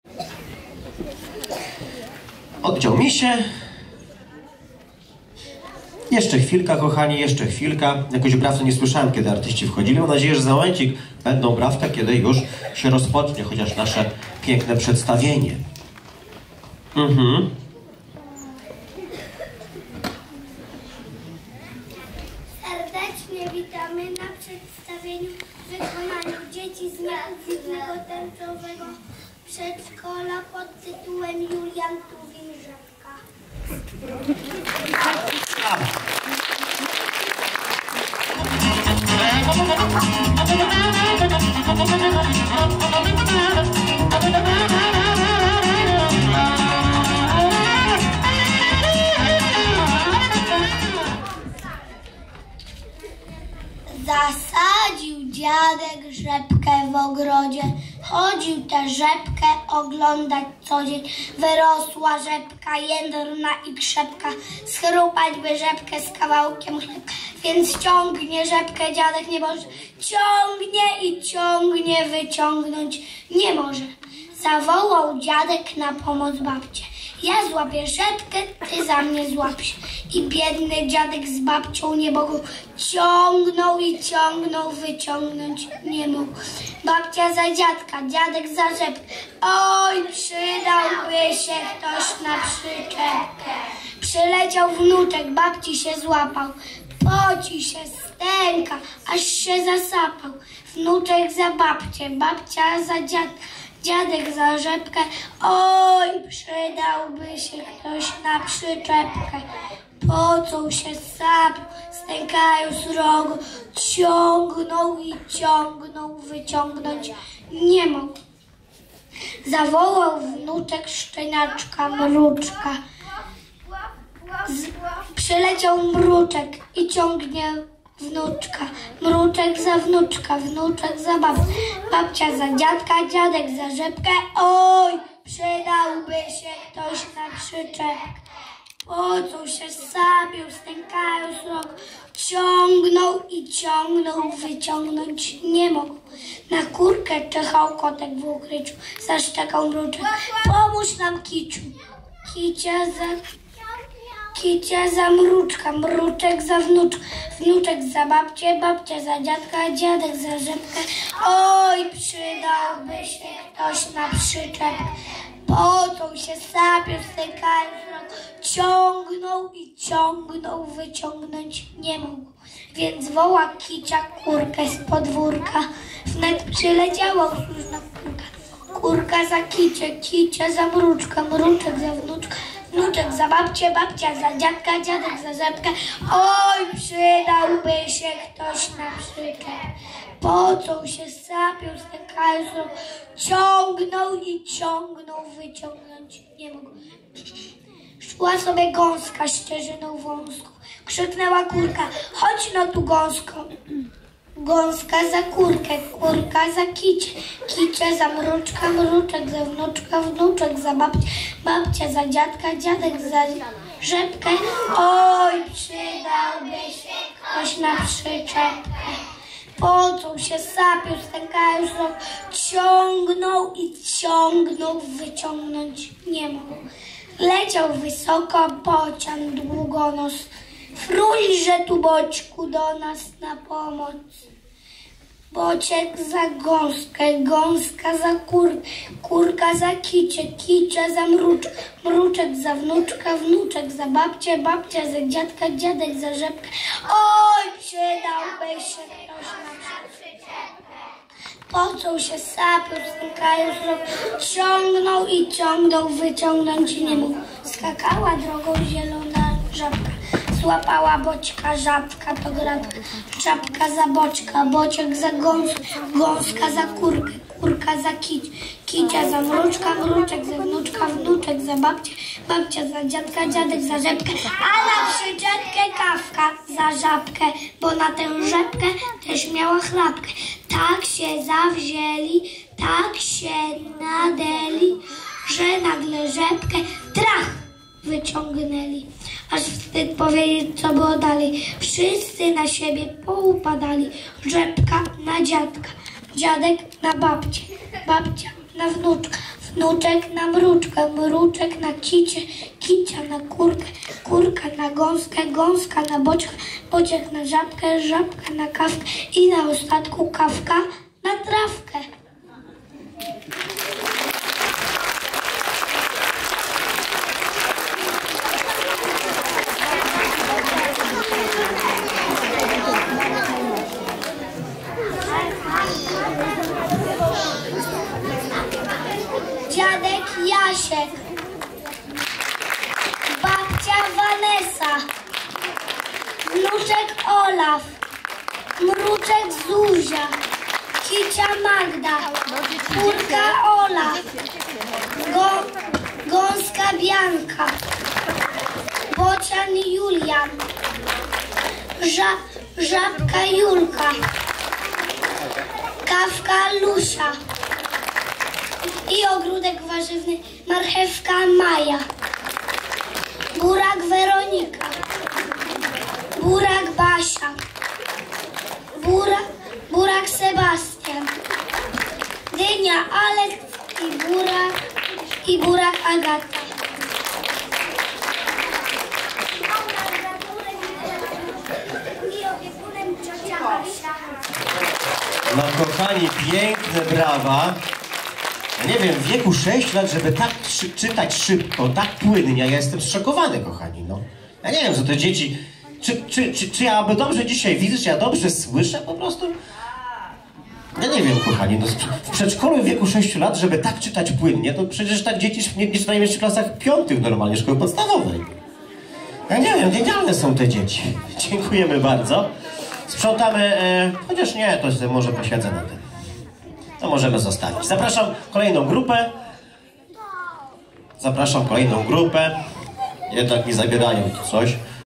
"Rzepka" Julian Tuwim
II Sochaczewski Festiwal Teatralny SPAT i SPAT-cik 2018
wykonanie grupa Misiów z Tęczowego Przedszkola w Sochaczewie